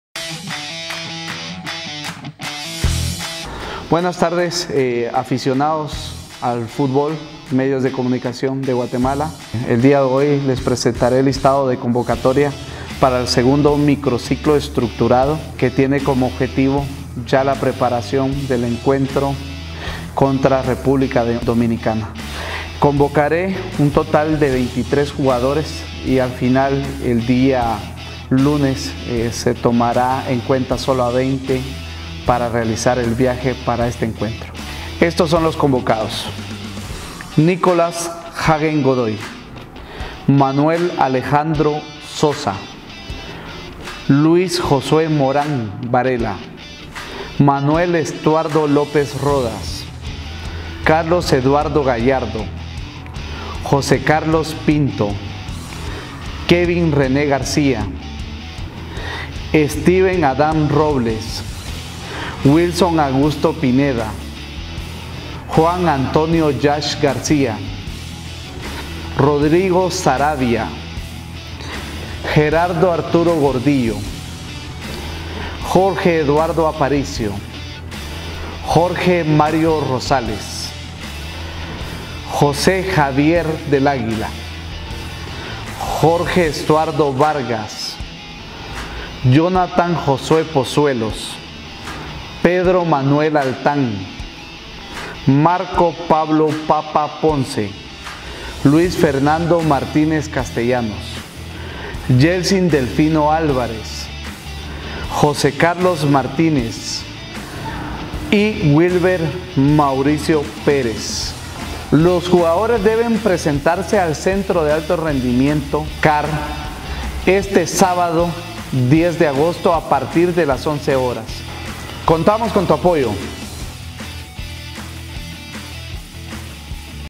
Convocados para el partido Guatemala vs República Dominicana.
El Director Técnico de la Selección Mayor Masculina de Fútbol, Amarini Villatoro, dio a conocer la nómina de 23 jugadores que convocará para prepararse de cara al partido amistoso ante República Dominicana el próximo 14 de agosto a las 17:00 horas local en el Estadio Cibao FC de la isla.